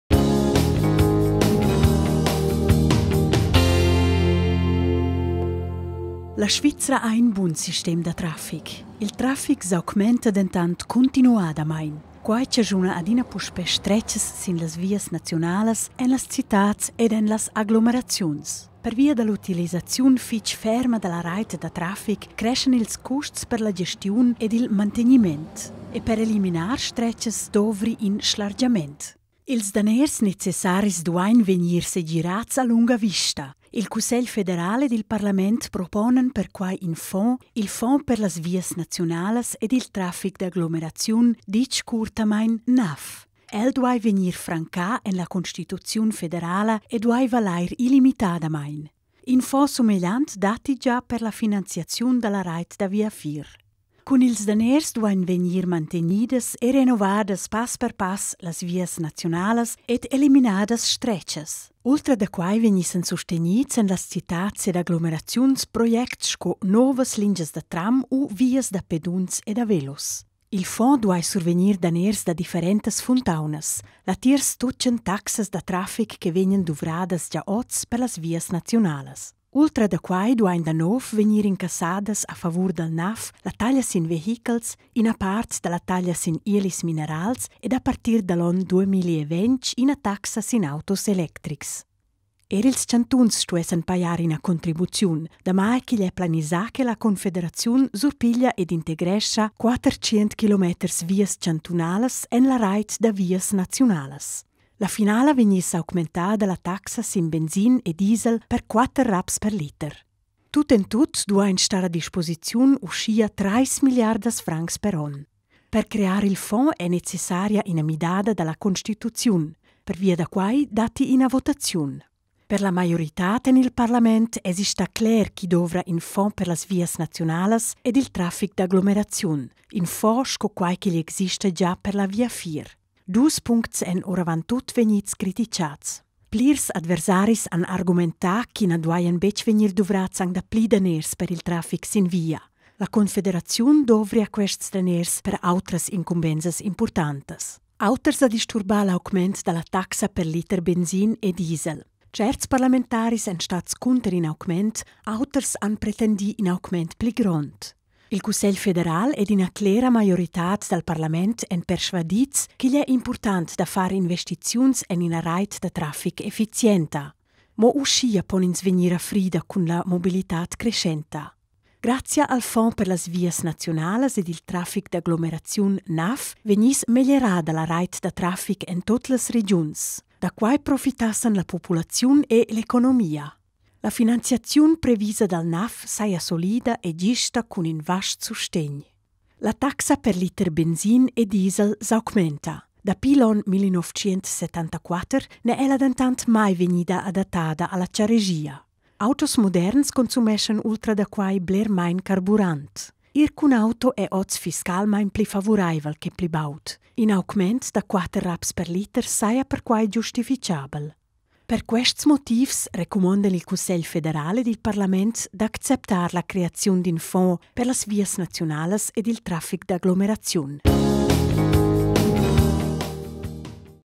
Fond per las vias naziunalas ed il traffic d’aglomeraziun (NAF): Votaziun dals 12.2.2017
Il pli impurtant en furma concisa or dal carnet da votar davart il project da votaziun «conclus federal davart la creaziun d’in fond per las vias naziunalas ed il traffic d’aglomeraziun (NAF)».  Votaziun dal pievel dals 12 da favrer 2017.  Ils suttitels pon vegnir activads directamain en il video.